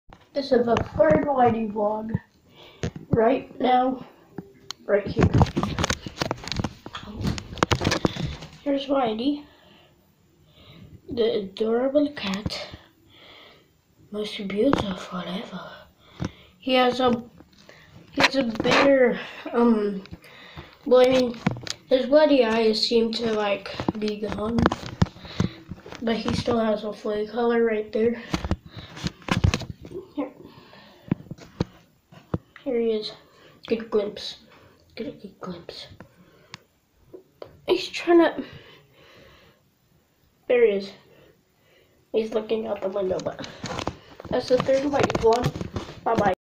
Whitey Vlog #3 Update?
Whitey Has a bloody eye but it's getting way better than it used to but he still has a flea collar. Tablet: Atlas 10 Pro With Attachable Keyboard That Includes H
eadphones